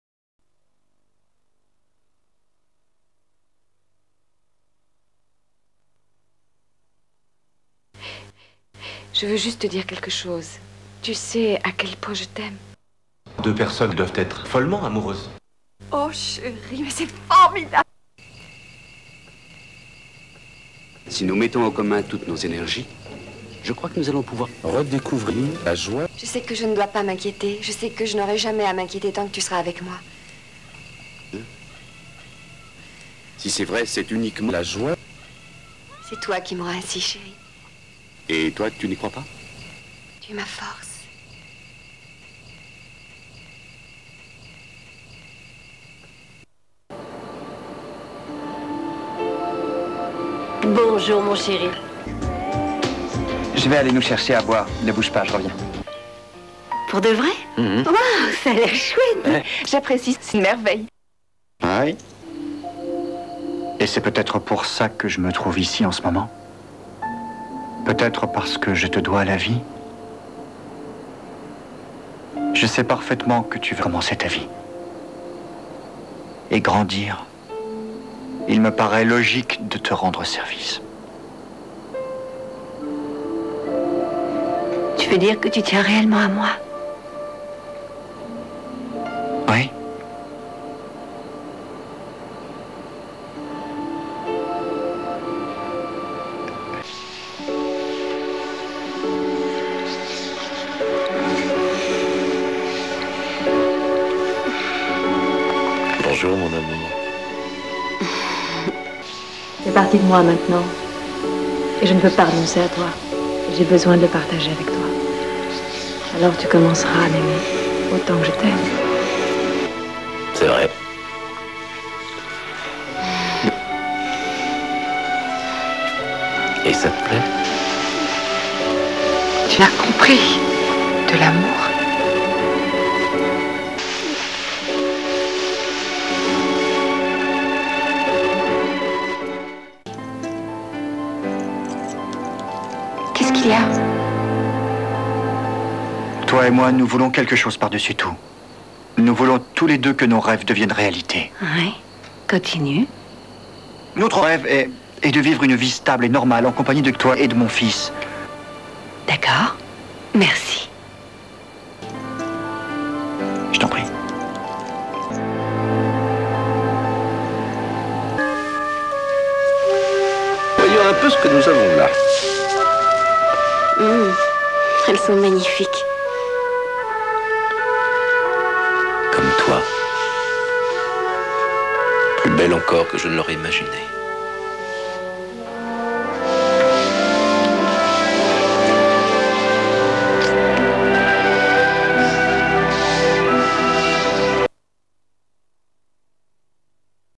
que du bonheur, version complète.
Série de scénettes, collage à propos du couple et de l'amour.

Quand il s'est agit de diffuser cette série (avril à juin 2001) dans les festivals, j'ai compilé tous les épisodes dans un format compact.

Et même si j'avais créé cette série pour servir de virgules indépendantes, que le but était aussi de profiter du P2P pour les diffuser (donc avec un poids léger), je dois dire que j'aime cette compilation et que la narration que j'ai dû créé pour diffuser ce petit film me plait vraiment beaucoup.